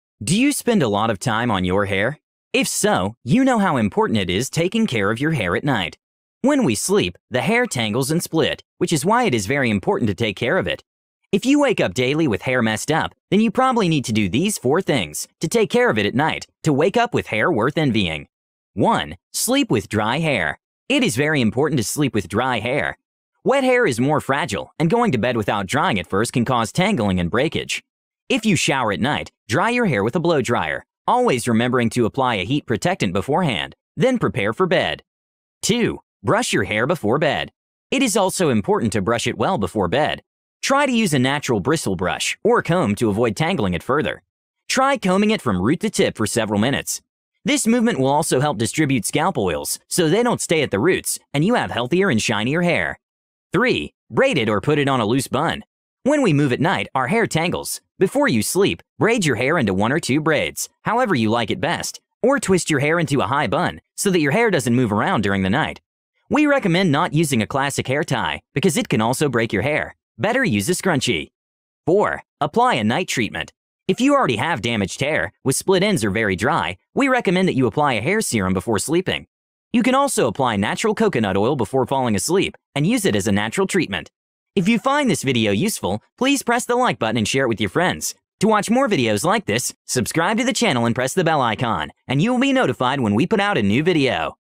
How To Sleep On Your Hair & Stop Breakage | Top 4 How To Sleep On Your Hair & Stop Breakage
If so you know how important it is taking care of your hair at night.

When we sleep, the hair tangles and split, which is why it is very important to take care of it.

If you wake up daily with hair messed up, then you probably need to do these 4 things to take care of it at night to wake up with hair worth envying.

1. Sleep with dry hair
It is very important to sleep with dry hair.

Wet hair is more fragile and going to bed without drying it first can cause tangling and breakage.

If you shower at night, dry your hair with a blow dryer - always remembering to apply a heat protectant beforehand - then prepare for bed.

2. Brush your hair before bed

It is also important to brush it well before bed.

Try to use a natural bristle brush or comb to avoid tangling it further.

Try combing it from root to tip for several minutes.

This movement will also help distribute scalp oils so they don’t stay at the roots and you have healthier and shinier hair.

3. Braid it or put it on a loose bun

When we move at night, our hair tangles.

Before you sleep, braid your hair into one or two braids, however, you like it best, or twist your hair into a high bun so that your hair doesn’t move around during the night.

We recommend not using a classic hair tie, because it can also break your hair. Better use a scrunchie.

4. Apply a night treatment

If you already have damaged hair, with split ends or very dry, we recommend that you apply hair serum before sleeping.

You can also apply natural coconut oil before falling asleep and use it as a natural treatment.